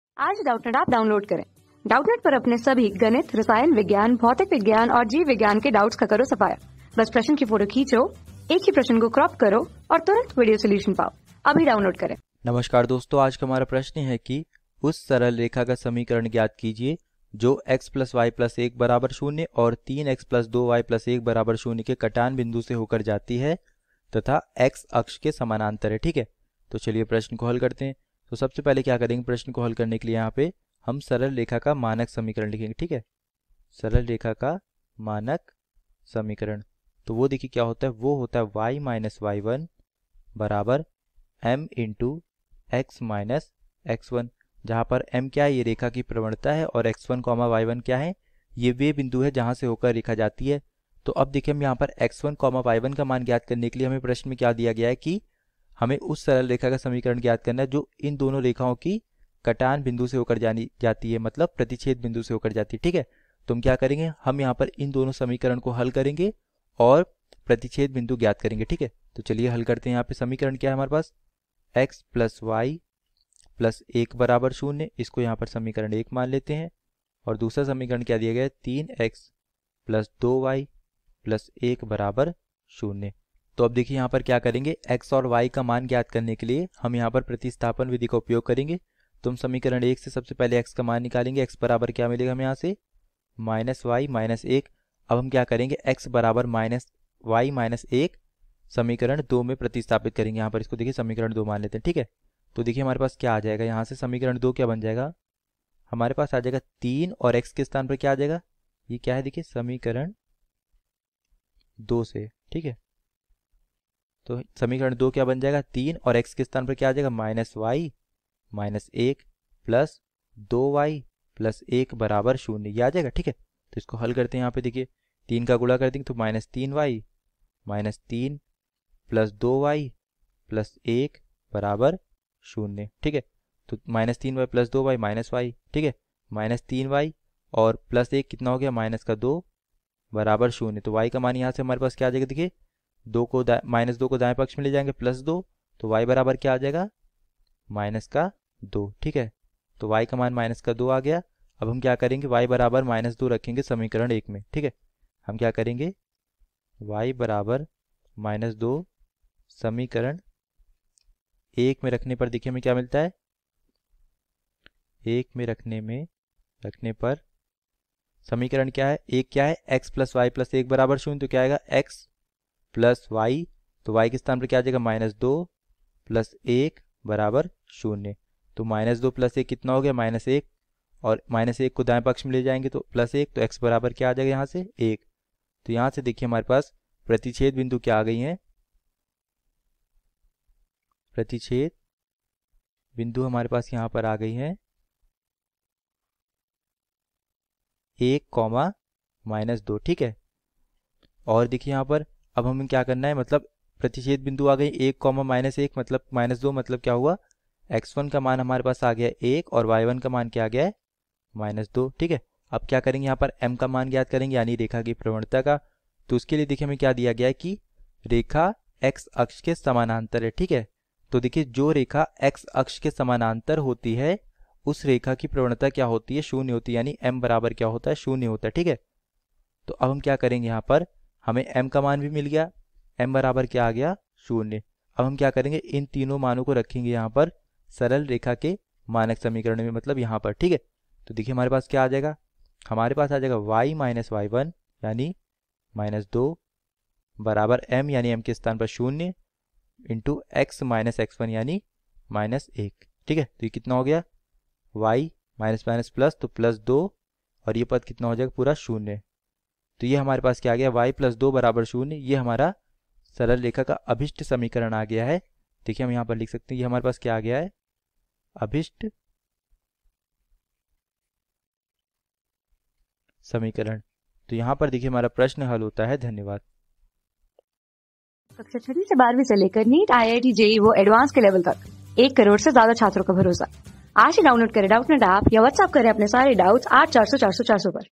उस सरल रेखा का समीकरण ज्ञात कीजिए जो x+y+1=0 और 3x+2y+1=0 के कटान बिंदु से होकर जाती है तथा X-अक...
उस सरल रेखा का समीकरण ज्ञात कीजिए जो x+y+1=0 और 3x+2y+1=0 के कटान बिंदु से होकर जाती है तथा X-अक्ष के समान्तरहै ।
Class: 12
Subject: MATHS
Chapter: सरल रेखा
Board:Madhya Pradesh Board

👉 Have Any Query? Ask Us.